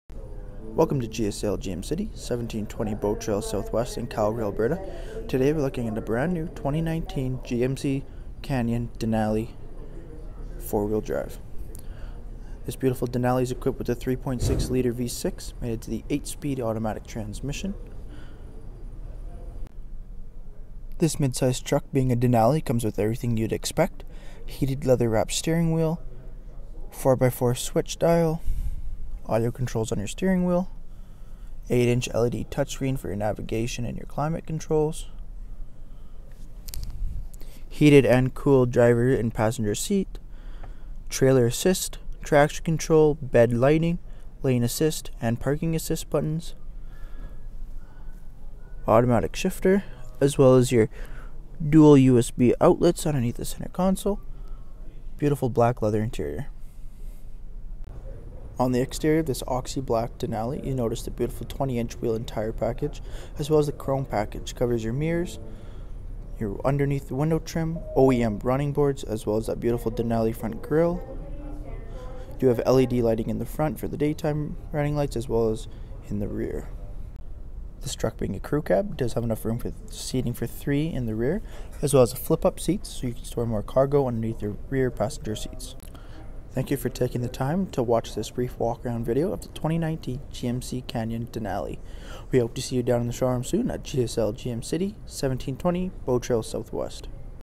Black 2019 GMC Canyon Denali Review - GSL GM City | Calgary
The new GMC Canyon Denali is a great utility truck for those you want a truck that can offer 4-wheel drive, a 3.6L V6 engine, and an automatic transmission. This truck demands attention with sleek body lines and sought-after Canyon design. The Denali model offers Denali styled alloy wheels, chrome housing headlamps, with halogen bulbs, LED daytime running lights and a pair of fog lamps. The grill is also of the Denali styling. The rear bumper sports a step to make access into the box easy. Equipped with a spacious cabin that offers bucket front seats, and standard options including power windows, power locks, power mirrors, cruise control, and automatic headlamps. The media zone is fun and intuitive, including options such as OnStar, rear-view camera, AM/FM radio, and standard Bluetooth for your phone. The features you design in a truck, are all wrapped up in a perfect combination of the Canyon. Stop in at 1720 Bow Trail SW Calgary, AB to see how it will check off everything on your wish list. *Pricing displayed may include lease discounts, OEM rebates, dealer discounts etc. See dealer for complete pricing details, | View photos and more info (Uploaded by DataDriver).